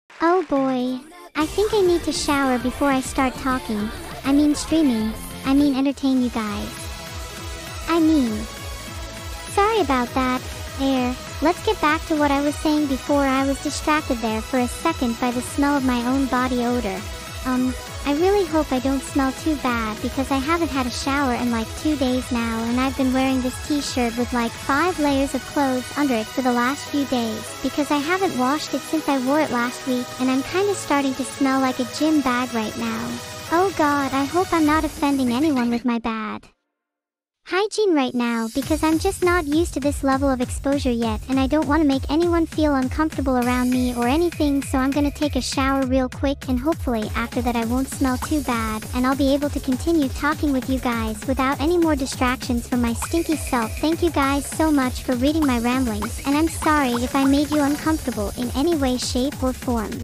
Neuro-sama is stinky, and goes on a rant about it
i dont know what to say xD